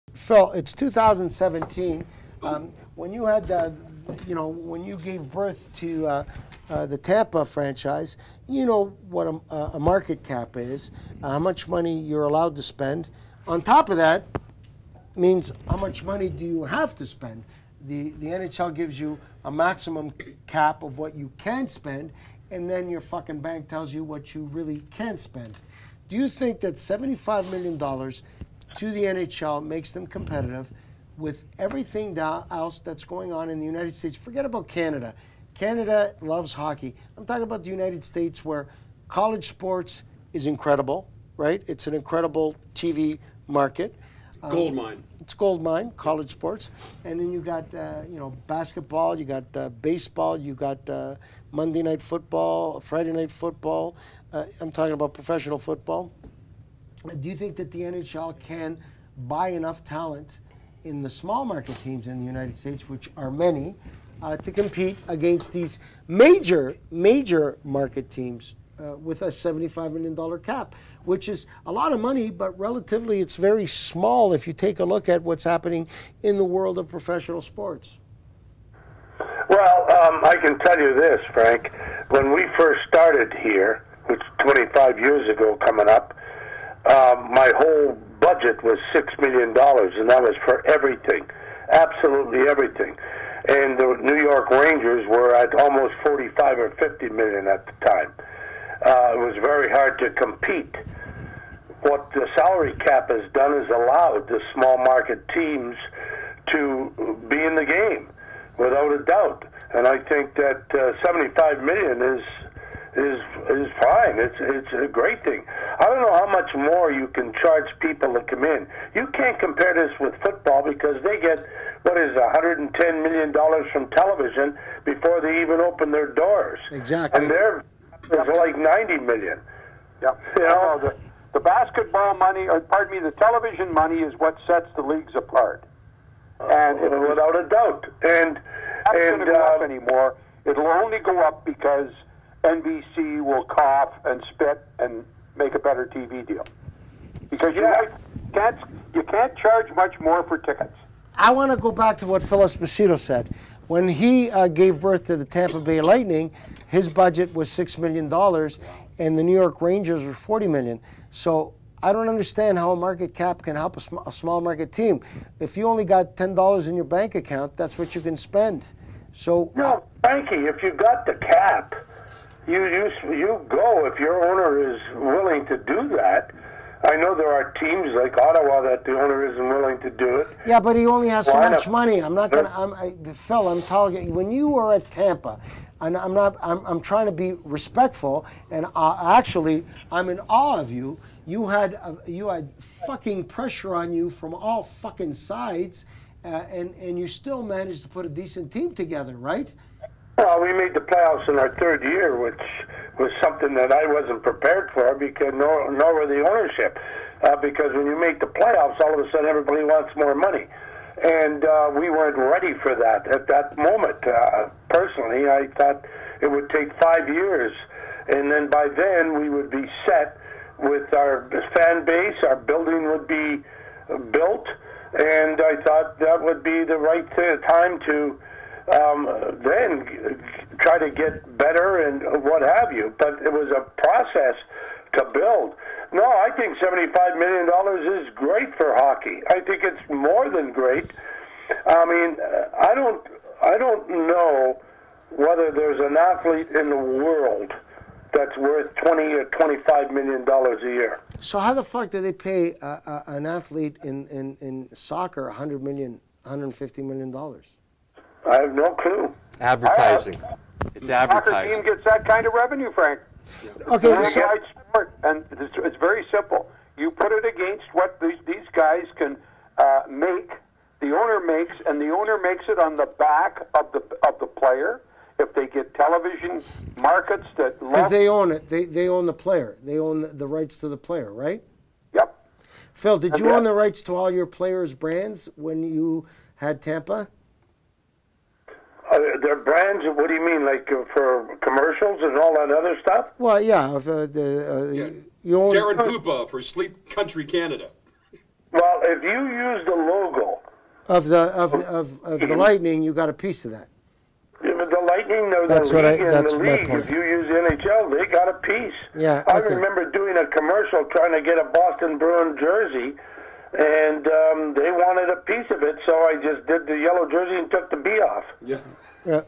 Does NHL salary cap need to be higher?
Frank D'Angelo and his panel of experts discuss the NHL's decision to set the salary cap at $75 million for the 2017/18 season.